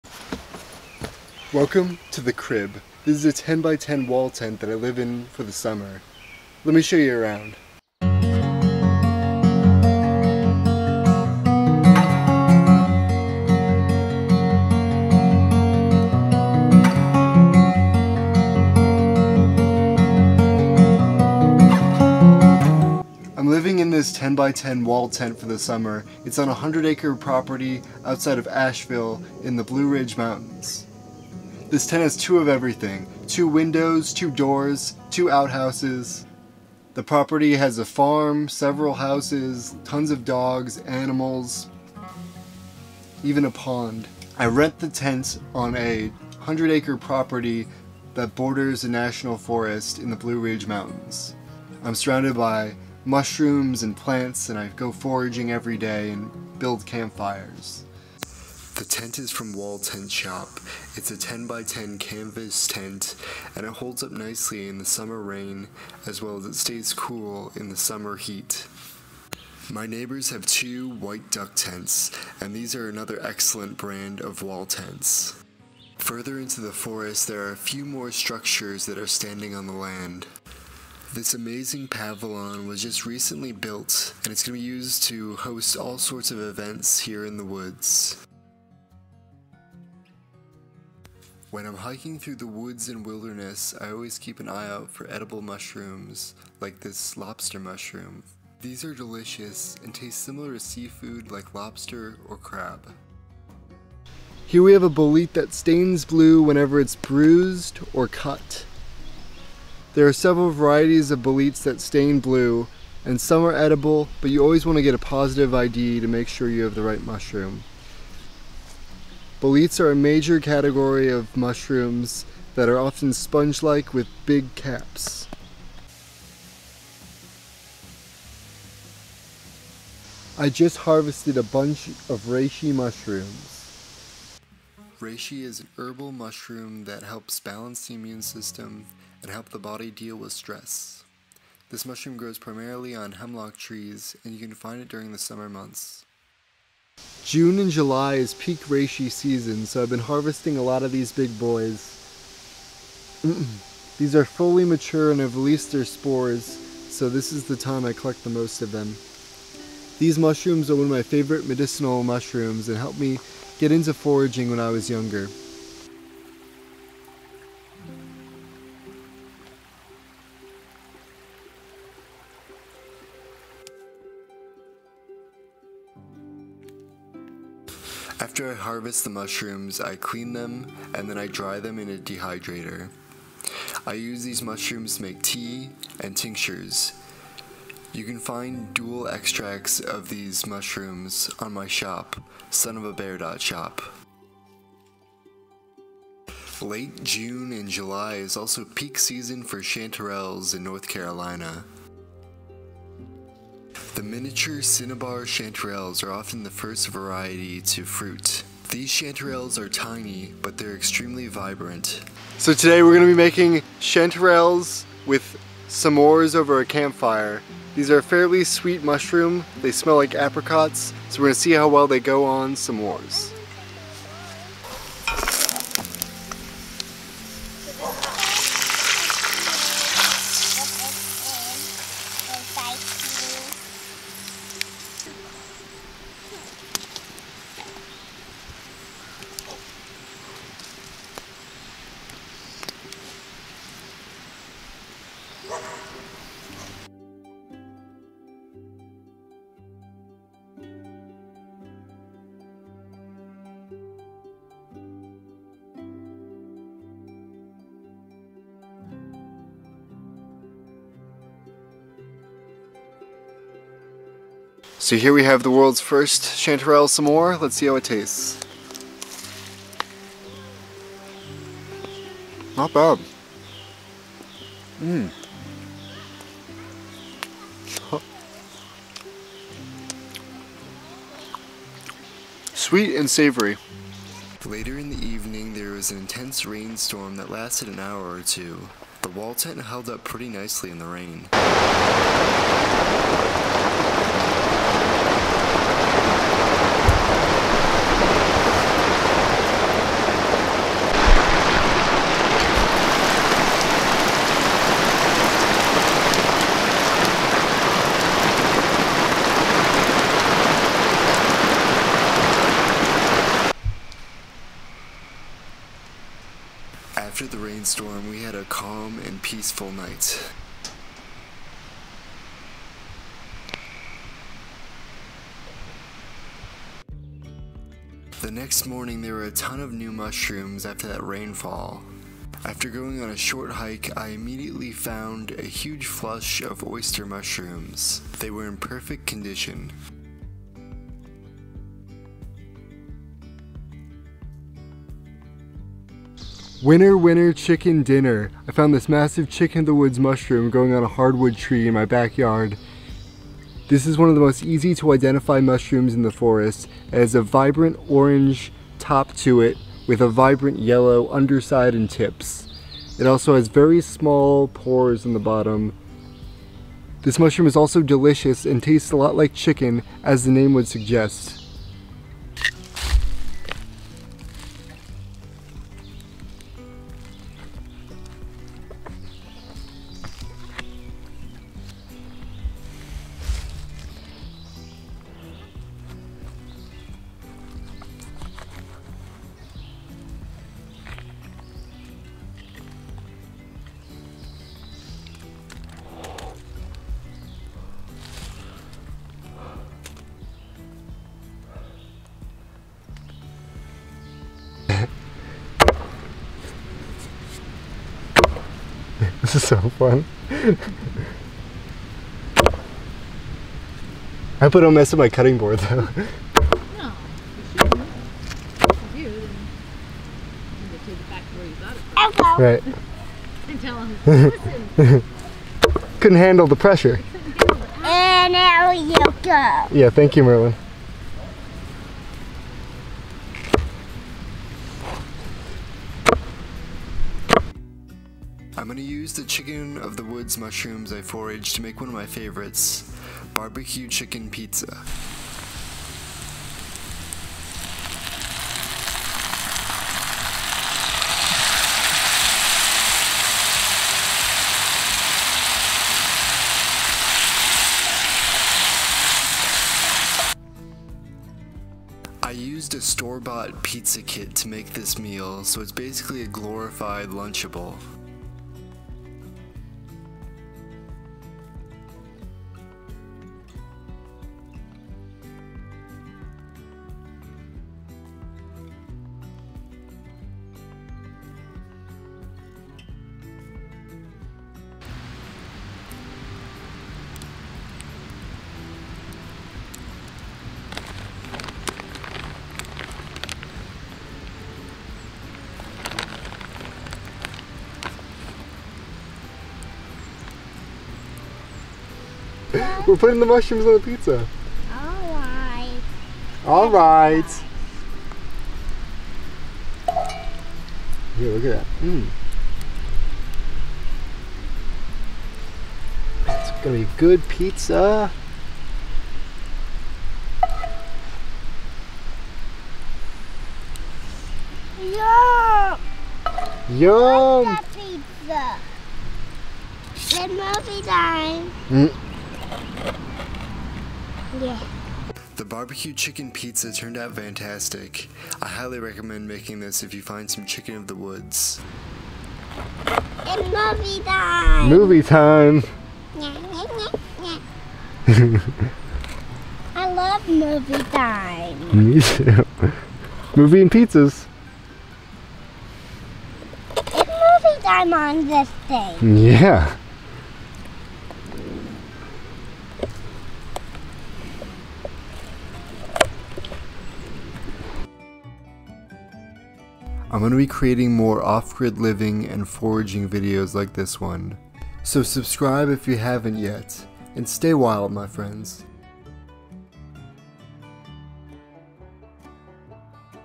Off Grid Wall Tent Life and Chicken of the Woods Mushroom Foraging. Reishi and Bushcraft. Fornite
I show my off the grid wall tent setup in the Blue Ridge Mountains, outside of Asheville, North Carolina. The tent is from Wall Tent and Pack Shop. It is called a 10 x 10 Spike Tent.

In the rest of the video, I forage numerous mushrooms like Lobster, Boletes, and Oysters.

I also find the motherload! A massive Chicken of the Woods mushroom and I cook over the campfire bushcraft style.

00:00 - Wall Tent and Homestead Tour
01:30 - Foraging Lobster Mushrooms and Boletes
02:06 - Wild harvesting Reishi Mushrooms
03:14 - Forage and Cook Chanterelle Mushrooms
04:43 - Thunderstorm in the Wall Tent
05:28 - Morning after the Storm
05:45 - Chicken of the Woods Mushrooms

My Drone
My Camera

Best Foraging Books